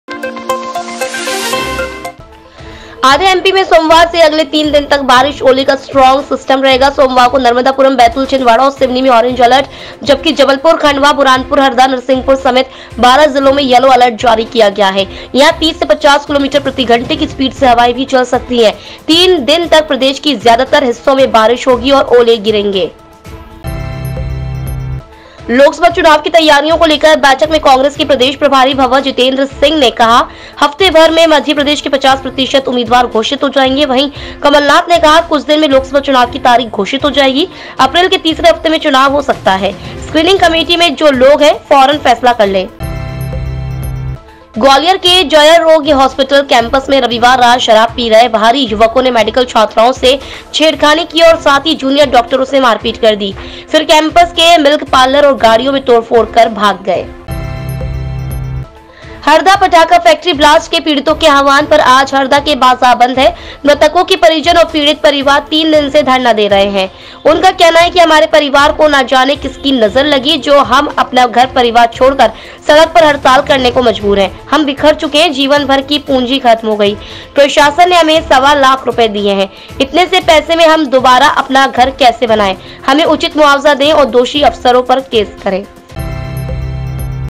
MP में बारिश-ओले का अलर्ट | EMS TV 24-Feb-2024
नर्मदापुरम, बैतूल, छिंदवाड़ा-सिवनी में बारिश-ओले का अलर्ट

आधे एमपी में सोमवार से अगले 3 दिन तक बारिश-ओले का स्ट्रॉन्ग सिस्टम रहेगा। सोमवार को नर्मदापुरम, बैतूल, छिंदवाड़ा और सिवनी में ऑरेंज अलर्ट, जबकि जबलपुर, खंडवा, बुरहानपुर, हरदा, नरसिंहपुर समेत 12 जिलों में यलो अलर्ट जारी किया गया है। यहां 30 से 50 Km प्रति घंटे की स्पीड से हवाएं भी चल सकती हैं। 3 दिन तक प्रदेश के ज्यादातर हिस्सों में बारिश होगी और ओले गिरेंगे।

हफ्तेभर में आधे उम्मीदवार घोषित कर सकती है कांग्रेस

लोकसभा चुनाव की तैयारियों को लेकर बैठक में कांग्रेस के प्रदेश प्रभारी भंवर जितेंद्र सिंह ने कहा, 'हफ्ते भर में मध्यप्रदेश के 50% उम्मीदवार घोषित हो जाएंगे।' वहीं, कमलनाथ ने कहा, 'कुछ दिन में लोकसभा चुनाव की तारीख घोषित हो जाएगी। अप्रैल के तीसरे हफ्ते में चुनाव हो सकता है। स्क्रीनिंग कमेटी में जो लोग हैं, फौरन फैसला कर लें।

ग्वालियर JAH में शराबियों ने मेडिकल छात्राओं को छेड़ा

ग्वालियर के जयारोग्य हॉस्पिटल (JAH) कैंपस में रविवार रात शराब पी रहे बाहरी युवकों ने मेडिकल छात्राओं से छेड़खानी की और साथी जूनियर डॉक्टरों से मारपीट कर दी। फिर कैंपस के मिल्क पार्लर और गाड़ियों में तोड़फोड़ कर भाग गए।

पटाखा फैक्ट्री ब्लास्ट पीड़ित तीन दिन से धरने पर

हरदा पटाखा फैक्ट्री ब्लास्ट के पीड़ितों के आह्वान पर आज हरदा के बाजार बंद हैं। मृतकों के परिजन और पीड़ित परिवार तीन दिन से धरना दे रहे हैं। उनका कहना है कि 'हमारे परिवार को न जाने किसकी नजर लगी, जो हम अपना घर-परिवार छोड़कर सड़क पर हड़ताल करने को मजबूर हैं, हम बिखर चुके हैं। जीवन भर की पूंजी खत्म हो गई। प्रशासन ने हमें सवा लाख रुपए दिए हैं। इतने से पैसे में हम दोबारा अपना घर कैसे बनाएं? हमें उचित मुआवजा दें और दोषी अफसरों पर केस करें।'